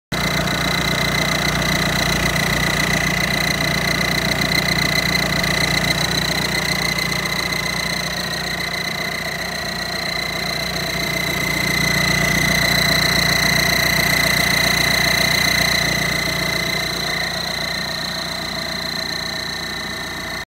Audi A6 EA189 Motor Geräusch - AGR undicht? Stehbolzen abgerissen, Spalt zwischen Krümmer und Block!
Ihr sehr und hört hier den Defekt an meinem Audi A6: Quietschen aus dem Motorraum; bzw.  vielleicht ein Pfeifen des Abgas-Rückführ-Ventils. Am Übergang des Rohrs in den Kühler scheint es, als ist die Naht undicht. Tatsächlich ist am Krümmer ein Stehbolzen abgerissen, und durch den Spalt zwischen Krümmer und Block kam das Abgas raus gepfiffen.